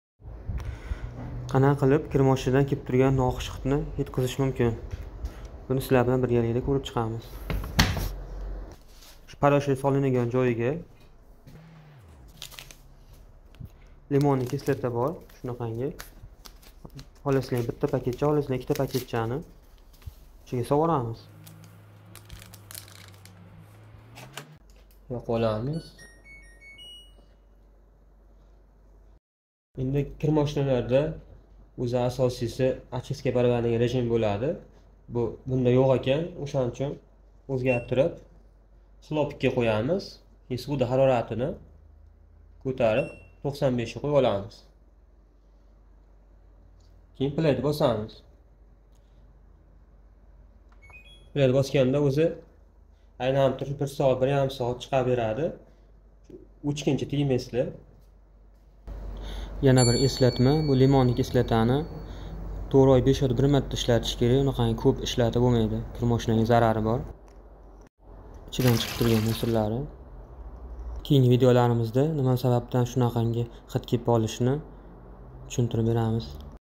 КИР ЮВИШ МАШИНАСИДАН НОХУШ ХИД КЕЛСА
Кир ювиш машинасида нохуш хид келса кандай килиб муаммони тугирлашни урганиб оласиз